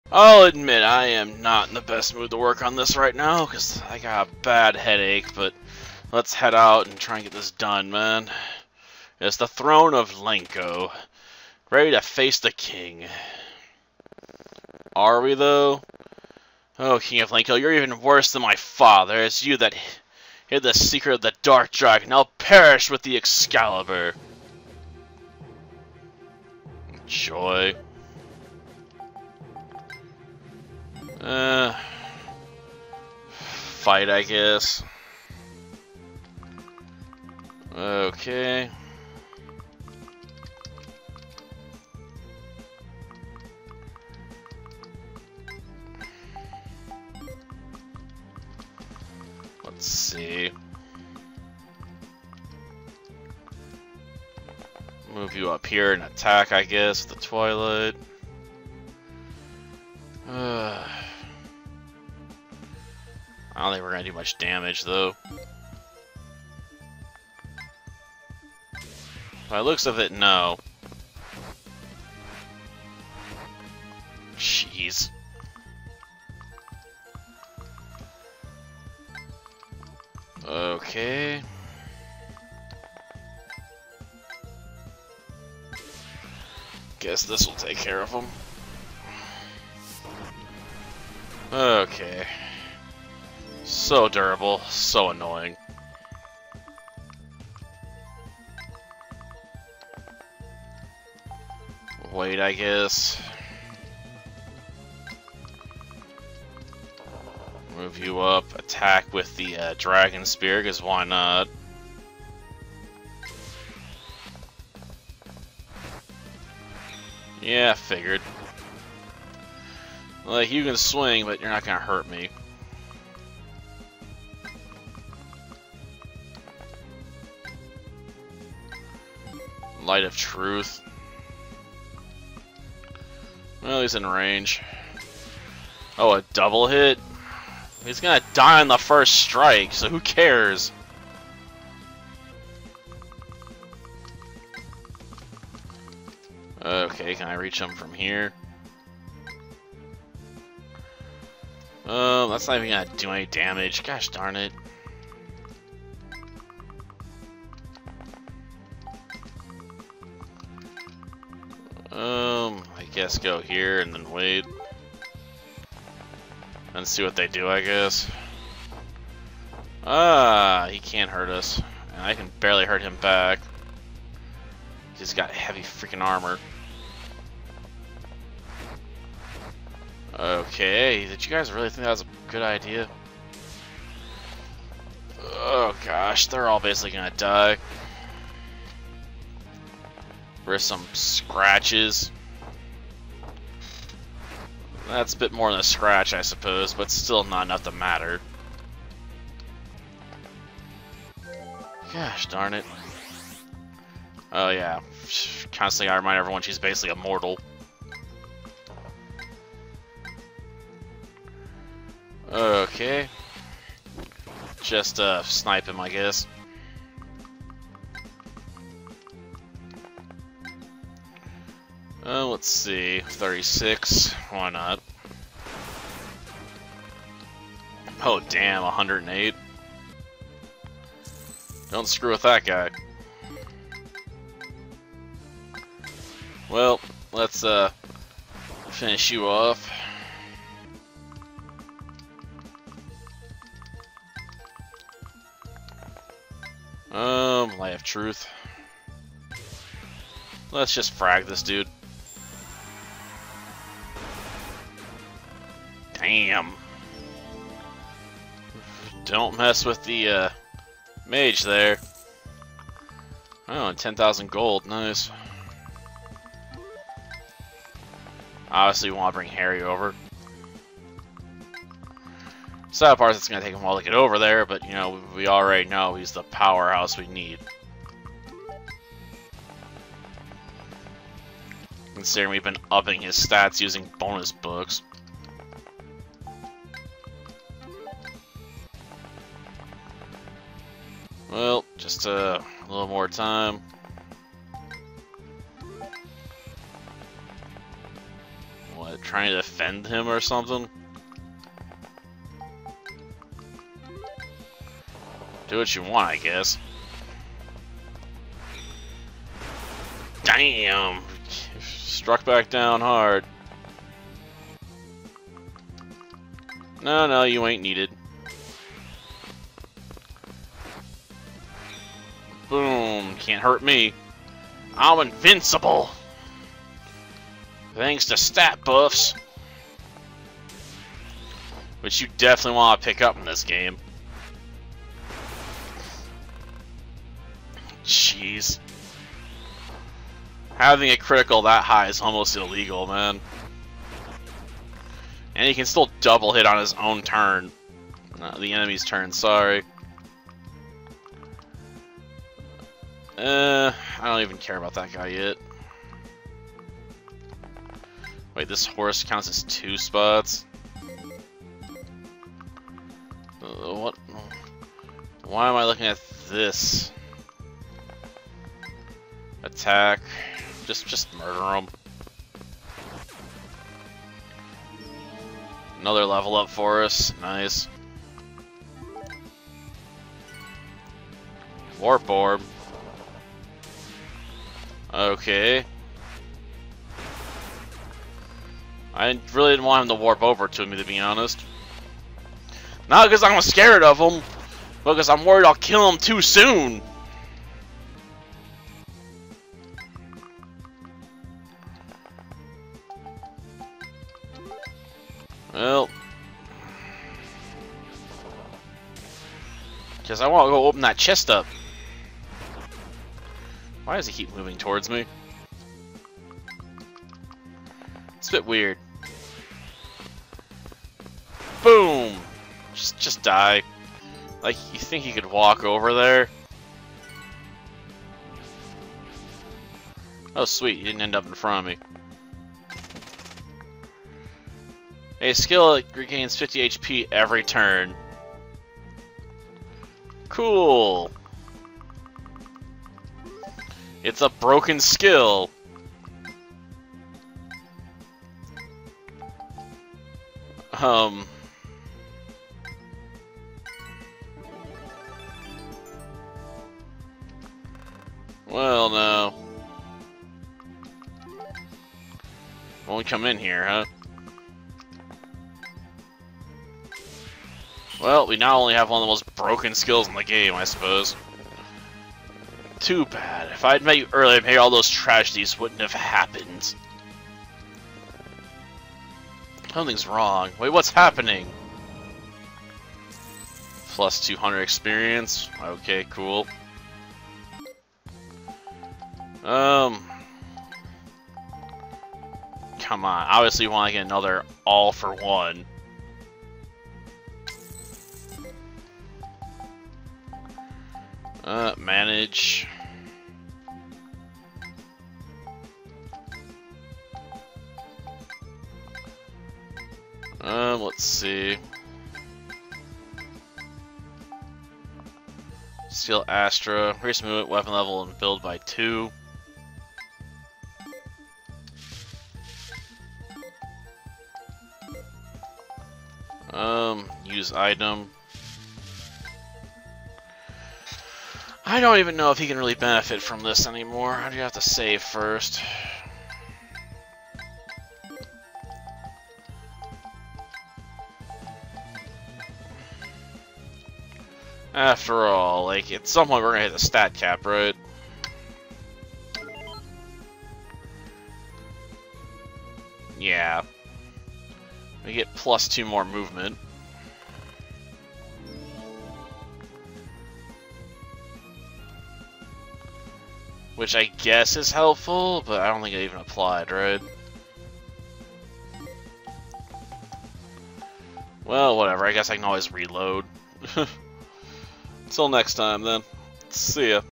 Legend of Cina - Part 10
In this episode we hunt down another king and gain a powerful new ability to make Lord Harry even more broken.